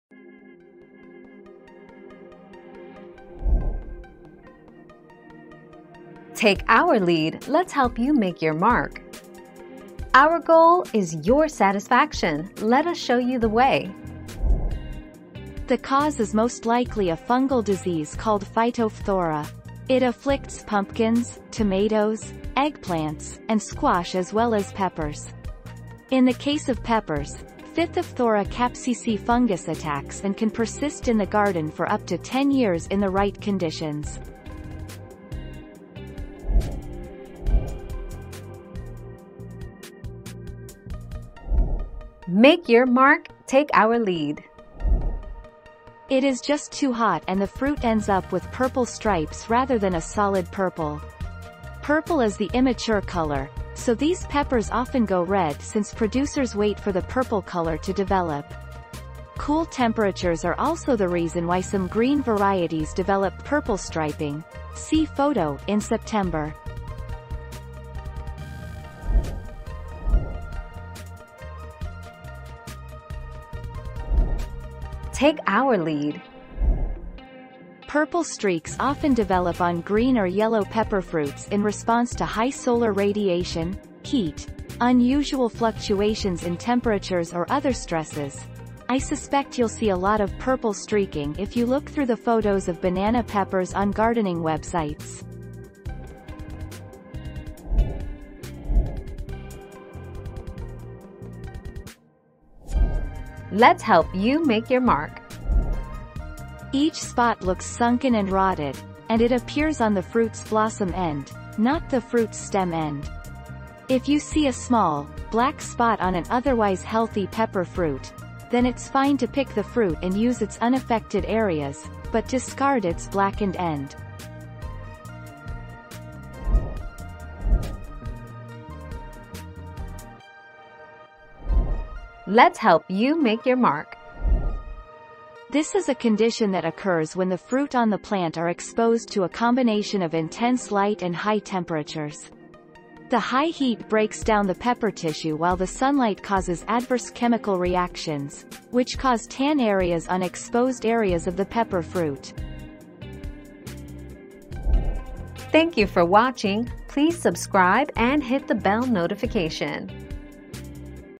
Why do my banana peppers have dark streaks?
00:00 - Why do my banana peppers have dark streaks?
00:38 - Why are my green peppers turning purple?
01:10 - Why are my tabasco peppers turning purple?
01:39 - Can I eat green peppers with black spots?
02:07 - Why did my peppers turn brown?

Laura S. Harris (2021, June 21.) Why do my banana peppers have dark streaks?
    AskAbout.video/articles/Why-do-my-banana-peppers-have-dark-streaks-255339